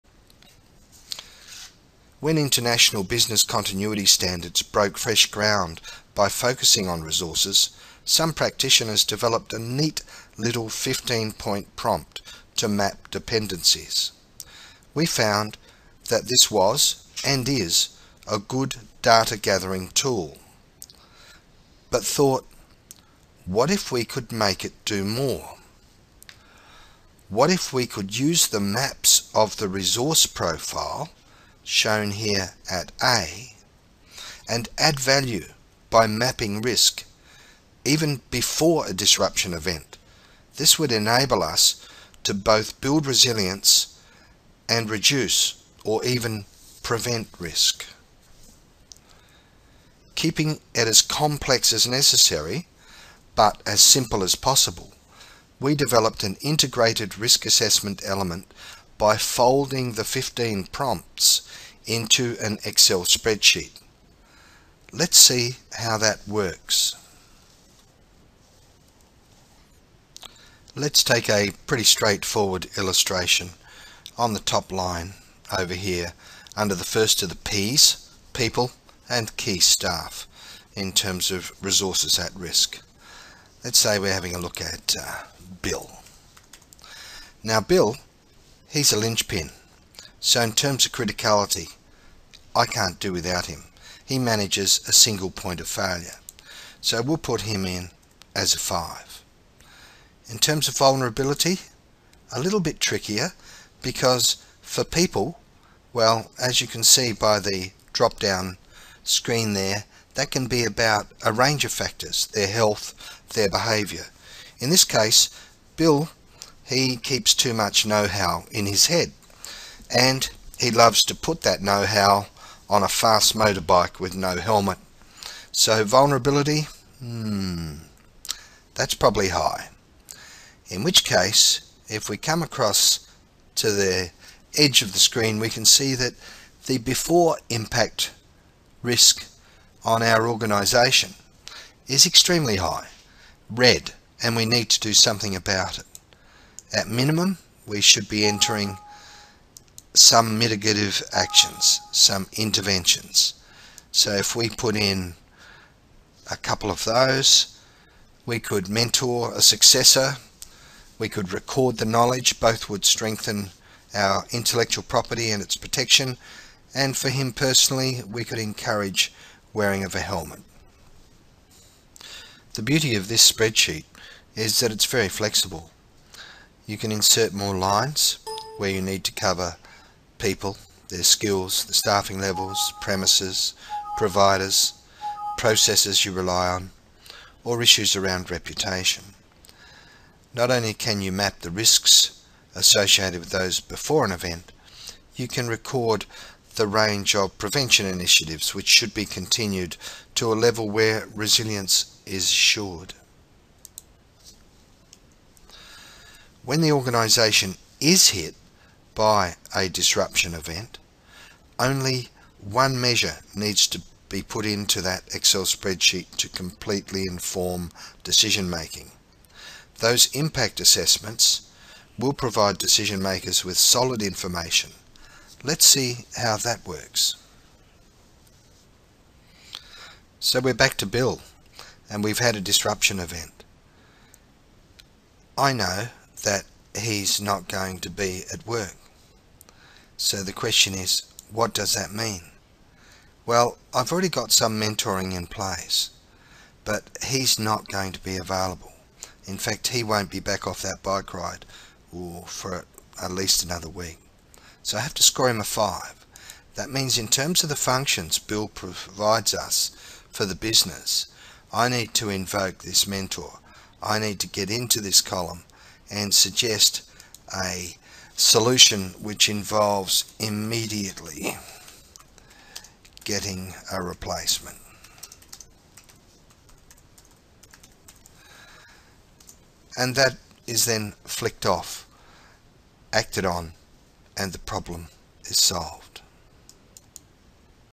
Business Impact Assessment Excel Tool
How to enter data into the risk based impact assessment tool before a crisis - to inform mitigation options.
How to enter data into the risk based impact assessment tool after an impact - to inform good decision making and priorities.